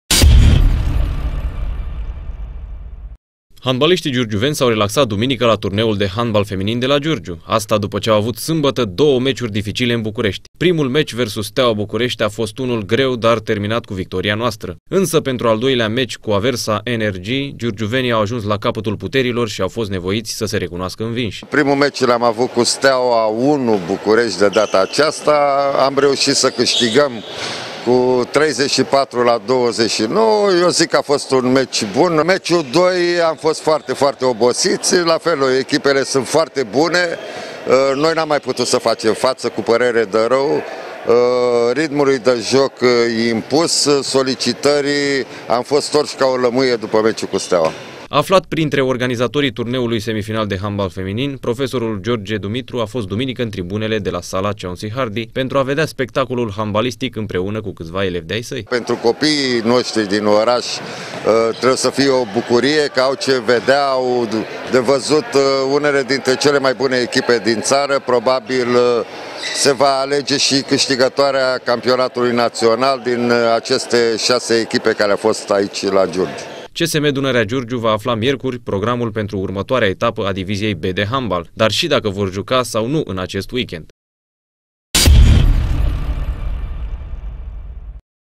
VICTORIE CU STEAUA
Echipa de handbal masculin a orașului, CSM Dunărea Giurgiu a câștigat sâmbătă meciul cu Steaua București în Divizia B. A fost primul meci disputat în acea zi și trăit la intensitate maximă de giurgiuveni. Puterile sportivilor noștri s-au epuizat la cel de-al doilea meci, pe care l-au pierdut.